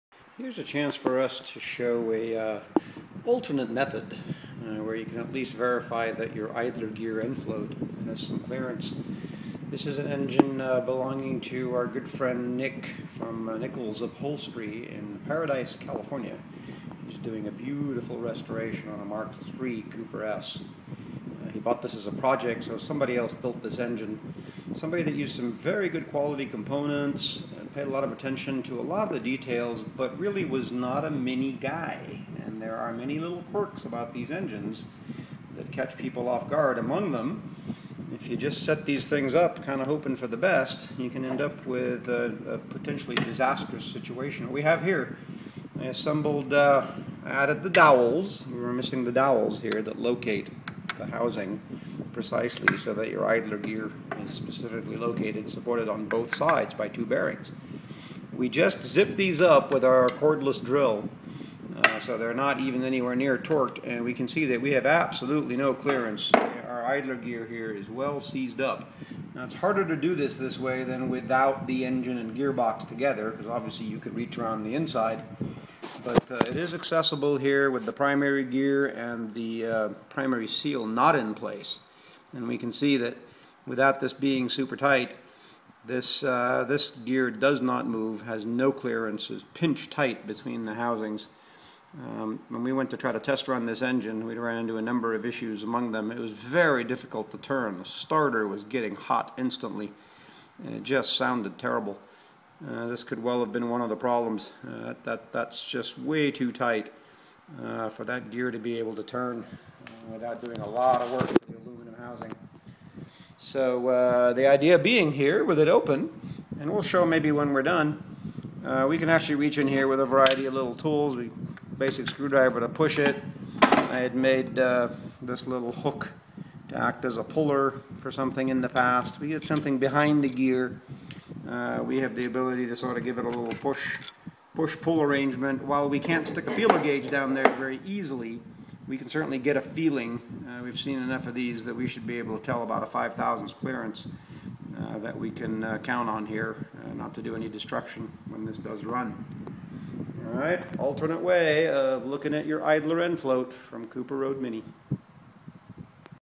How to verify idler end float for Classic Mini
Jemal shows a way to confirm your drop gears are set up right when someone else built the engine! No idler gear end float like we found here will destroy the transmission case and flywheel housing.